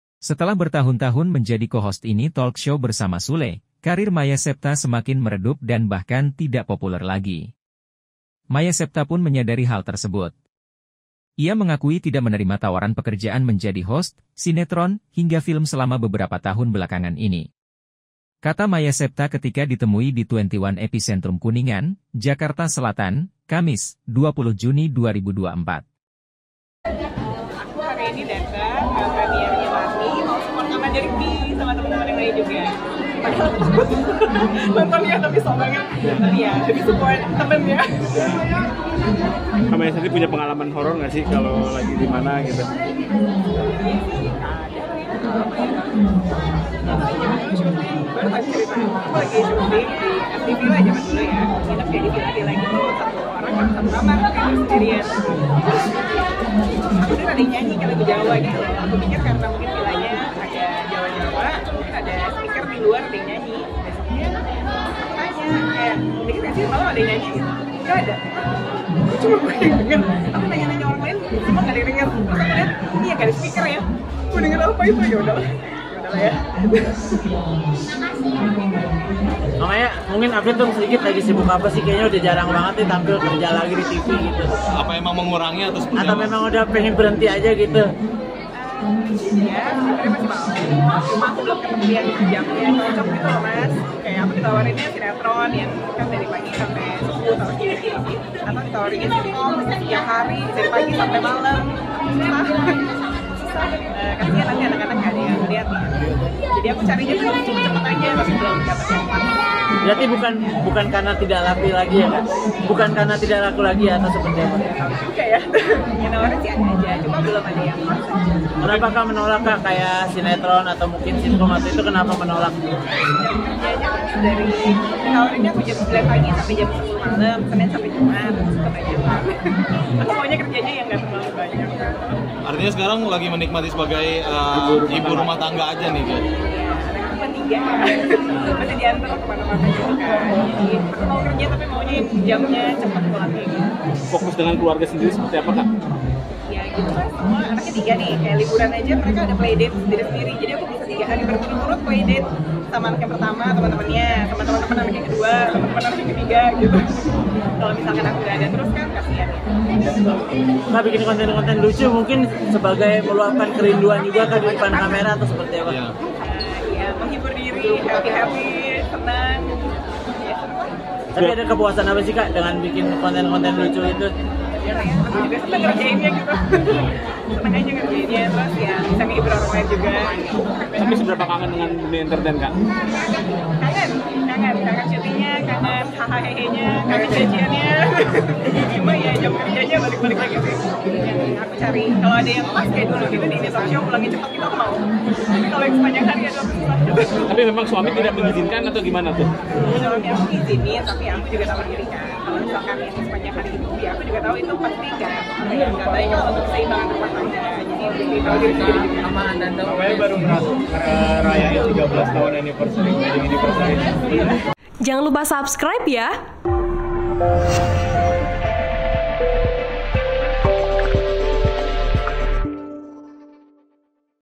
Demi Anak, Maya Septha Rela Tidak Tenar Lagi
WARTAKOTALIVE.COM, JAKARTA - Setelah bertahun-tahun menjadi co-host Ini Talk Show bersama Sule, karier Maya Septha semakin meredup dan bahkan tidak populer lagi.

Maya Septha pun menyadari hal tersebut. Ia mengakui tidak menerima tawaran pekerjaan menjadi host, sinetron, hingga film selama beberapa tahun belakangan ini.

"Alasannya tuh karena aku punya anak tiga," kata Maya Septha ketika ditemui di XXI Epicentrum Kuningan, Jakarta Selatan, Kamis (20/6/2024).

Maya tak menampik banyak sekali tawaran yang datang dari membawakan program acara, sinetron, hingga sitkom. Namun, ia banyak menolaknya.

"Aku engga bisa kalau kerjanya tuh butuh waktu yang panjang karena anak, jadi susah waktunya," ucap wanita berusia 37 tahun itu.